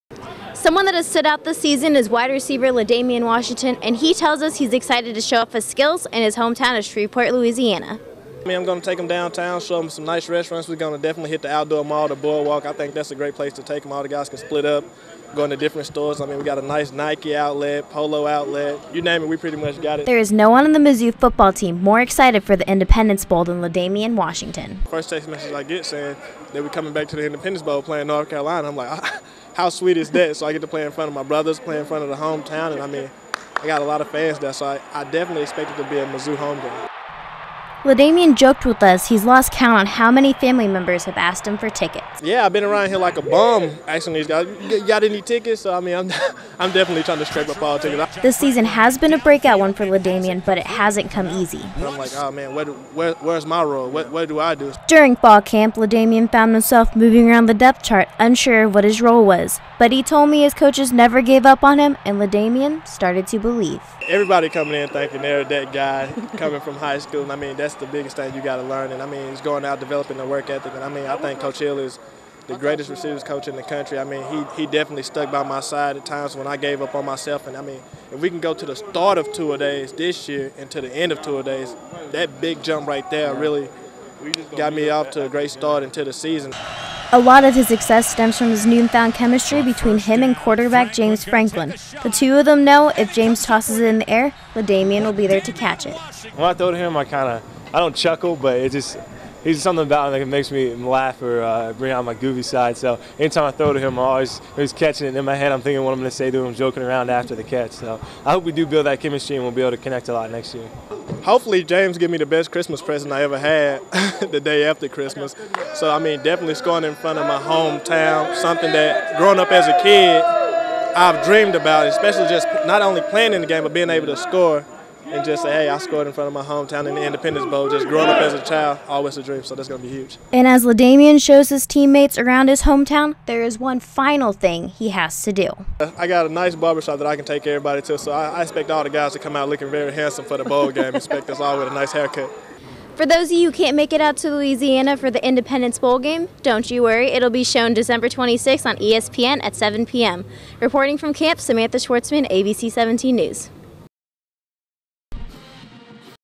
L'Damian Washington Package
A package I did about Mizzou sophomore WR L'Damian Washington and the Independence Bowl